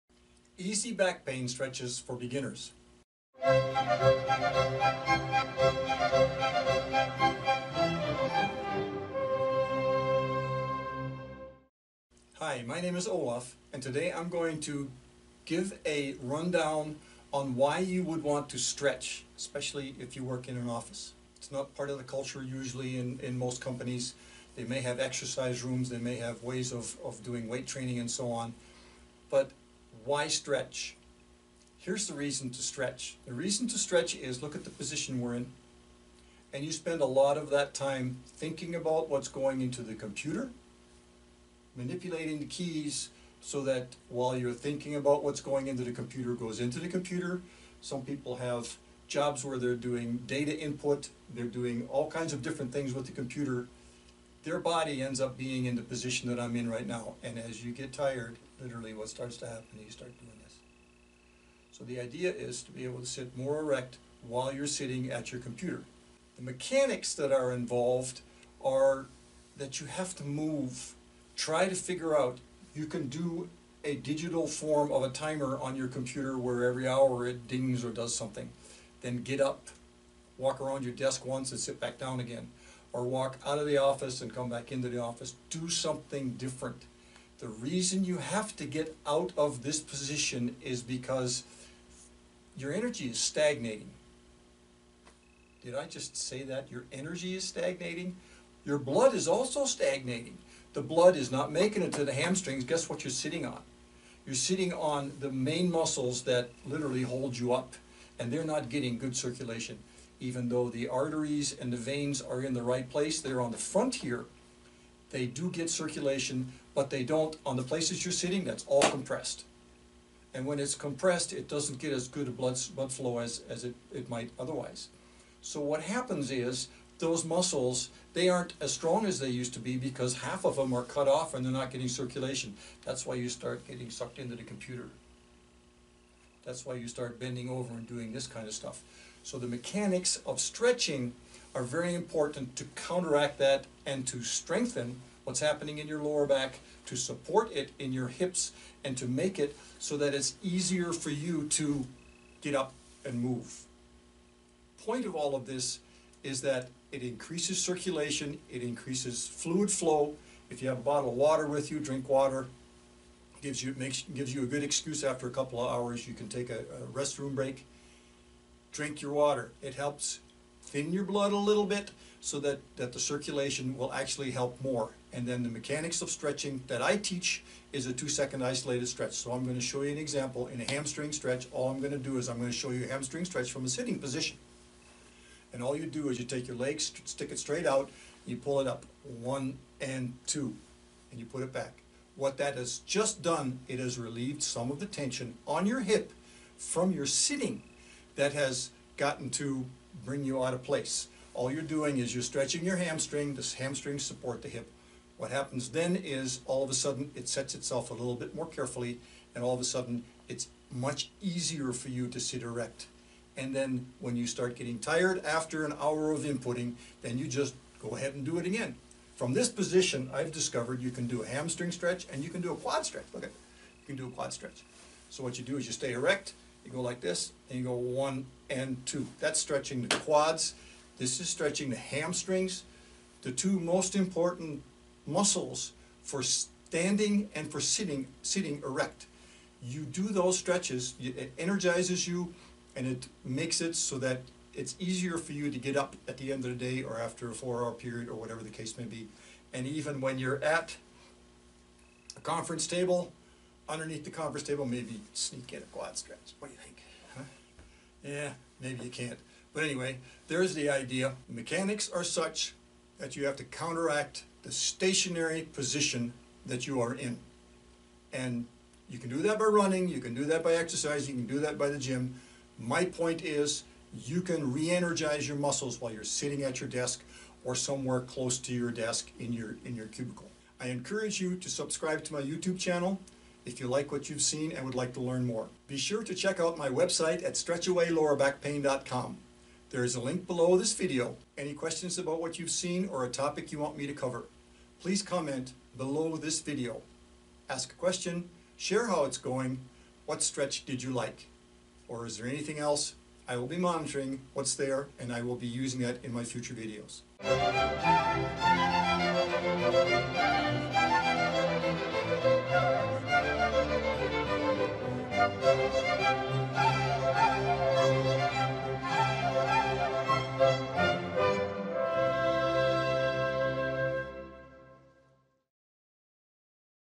Easy Back Pain Stretches for Beginners | Office & Desk Workers
Are you new to stretching? Then this video is for you!  I talk about why exactly your back hurts from sitting all day at your desk and show you a couple of simple stretches you can do while sitting.